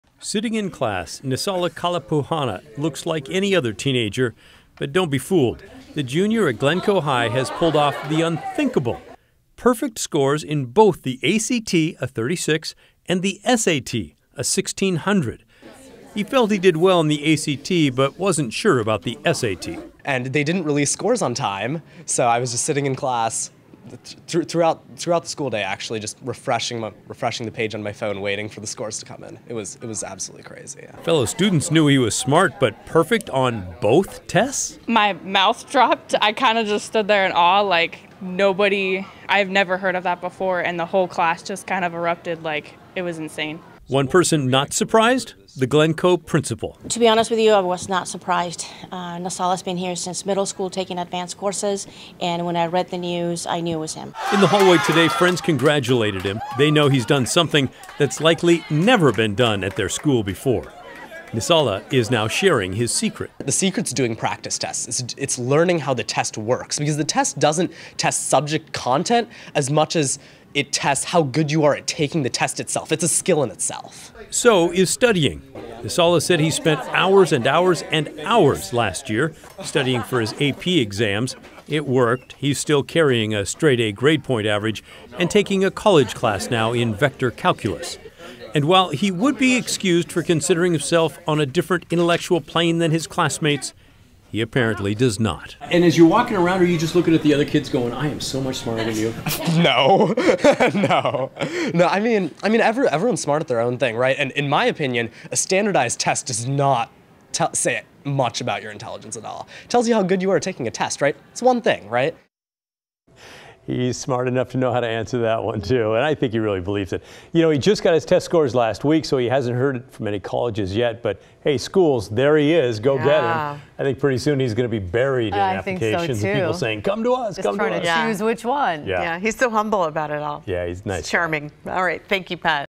Glencoe High School students gets perfect SAT, ACT scores
A students at Glencoe High School not only aced his SAT, but the ACT as well.